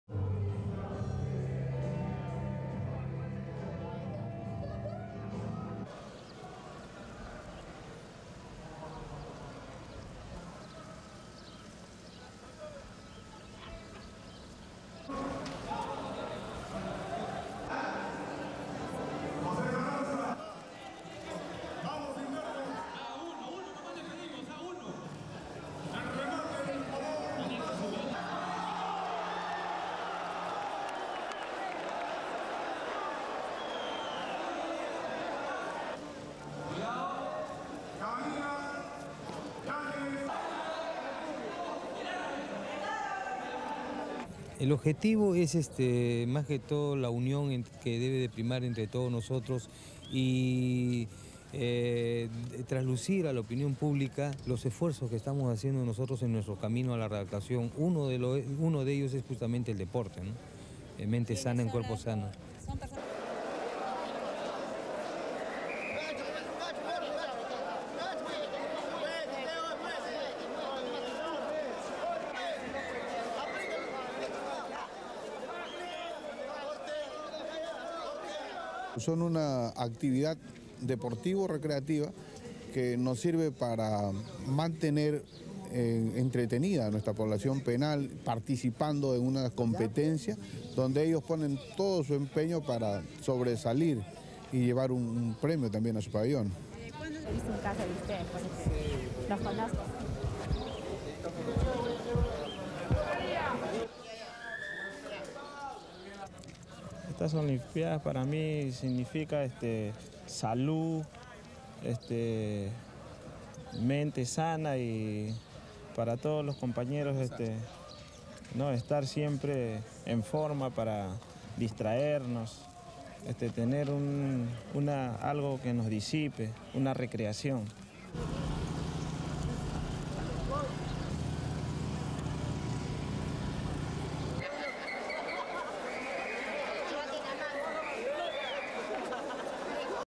PERU: SAN PEDRO PRISONERS TO HOLD THEIR OWN OLYMPIC GAMES
(17 Jul 1996) Spanish/Nat

Hundreds of Peruvian prison inmates can't make it to the Atlanta Olympic games - so they are holding their own.

The prisoners are waving their own Olympic flag and lighting an Olympic torch in the San Pedro Prison Olympics.

Despite the limitations of their facilities, for a month the inmates will  make the best of it.

The facilities might not be that good, but spirits are high.

Some 400 inmates in San Pedro prison, on the outskirts of Lima, are holding their own Olympic games from Tuesday.

Soccer, boxing, and baseball are the games expected to attract the most attention, but there are also individual games like chess, table tennis and athletics.

SOUNDBITE: (Spanish)
The aim is mostly to keep our unity and to communicate to the public the efforts we are making towards rehabilitation. One of the ways is through sports. A sound body makes a sound mind.
SUPER CAPTION: Carlos Chicoma, Prisoner Spokesman

Each wing of the prison has its own representatives and they compete against each other.

The director of the prison can only see advantages in their Olympics.

SOUNDBITE: (Spanish)
It's a recreational sports activity that helps us entertain the prisoners while they participate in the competition. The prisoners do their best to be outstanding and take the prize to their prison wing.
SUPER CAPTION: Col. Antonio Padilla, Prison Director

The idea has been so popular that they are thinking about having Olympic games every year.

SOUNDBITE: (Spanish)
To me, these Olympics mean health, a sound mind and a distraction for all my fellow inmates, to have something to think about
SUPER CAPTION: Guillermo, Prisoner

They may not get any gold medals here but they are getting plenty of satisfaction.